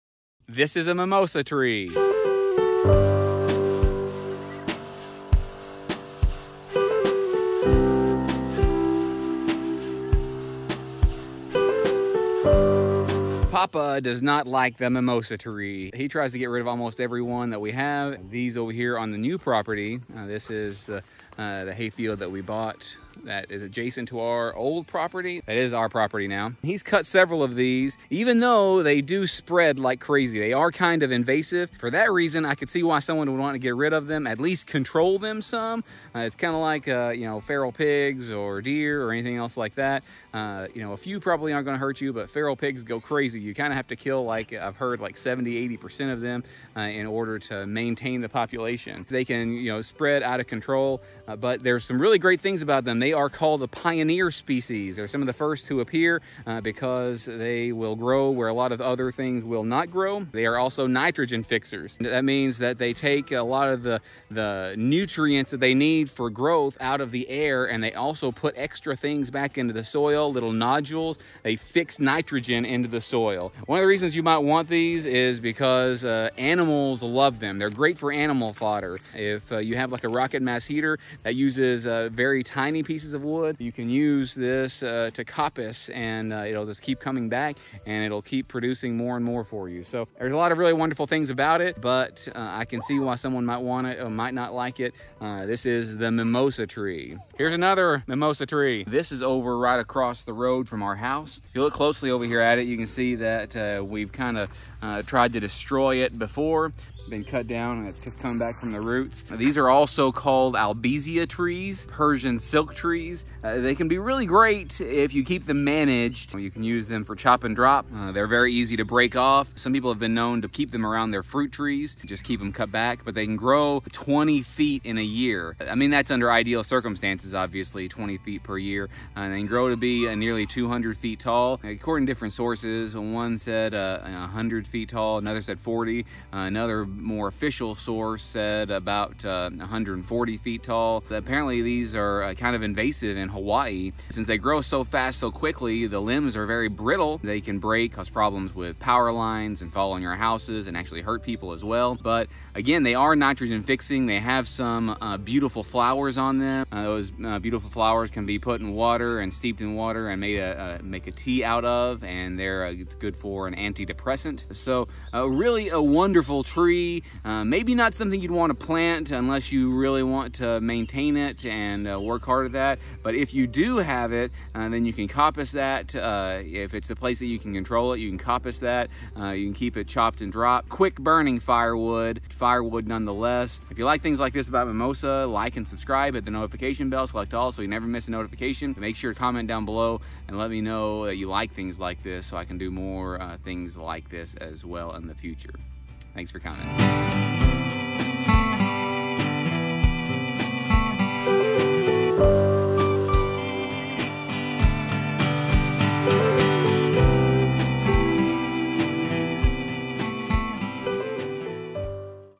Benefits & Invasiveness of Mimosa, Albizia, or Persian Silk Tree | Should You Grow It or Kill It?
Papa hates it, but along with its invasiveness there ARE benefits to the mimosa tree (also called albizia or persian silk tree). Should you grow it or kill it? What do you think?

COPPERHEAD ROAD HOMESTEAD: A place for preppers, homesteaders, nature-lovers, people who love their families, and EVERYONE!